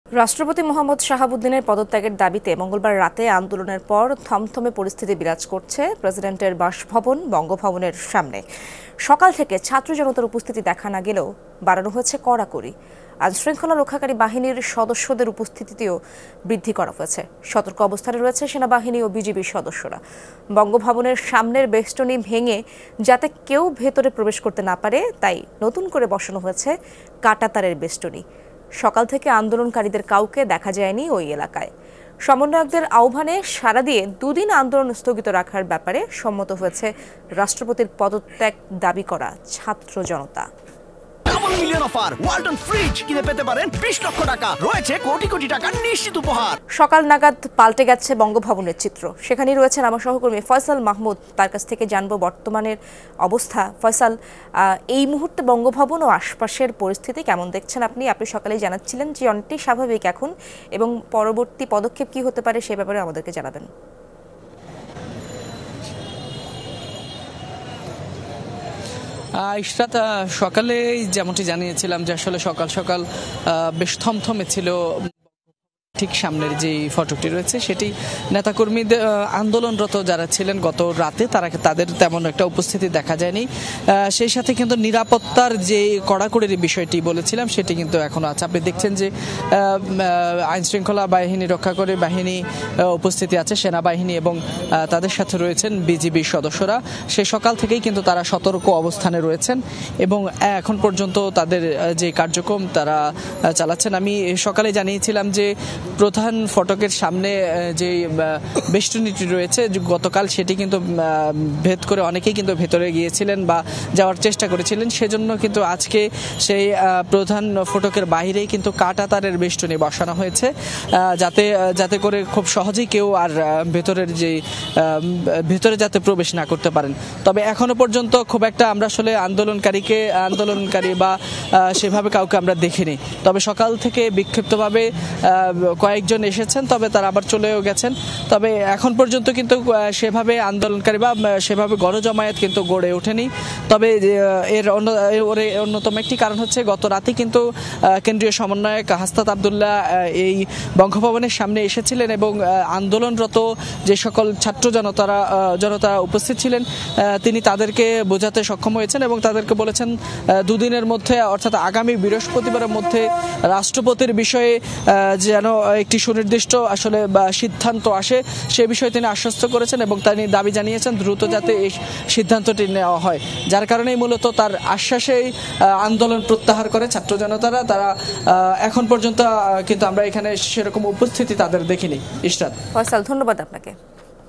আন্দোলনের পর থমথমে পরিস্থিতি বঙ্গভবনের সামনে | Bongovobon Situation | Jamuna TV
রাষ্ট্রপতি মো. শাহাবুদ্দিনের পদত্যাগের দাবিতে মঙ্গলবার রাতে আন্দোলনের পর; থমথমে পরিস্থিতি বিরাজ করছে প্রেসিডেন্টের বাসভবন- বঙ্গভবনের সামনে।